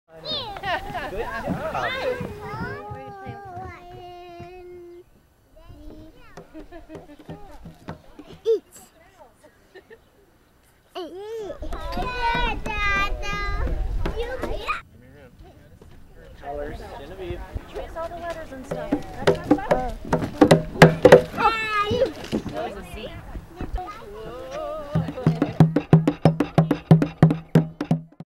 Bongo and Alphamaze Panel® - Weevos® - Landscape Structures
The Bongo and Alphamaze Panel® offers a bit of everything to kids ages 2 to 5. The bongo tones provide auditory stimulation while the Alphamaze offers tactile fun.